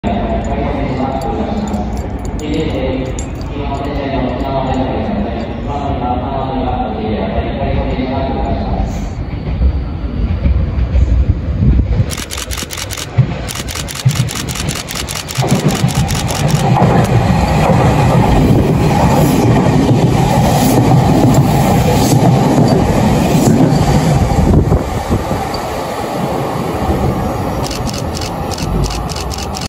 2024.3.27 南海本線8300系8308F千代田出場本線返却回送通過シーン
編成:8308F

車外カメラを取り付け、検査はせずに本線へ返却されました。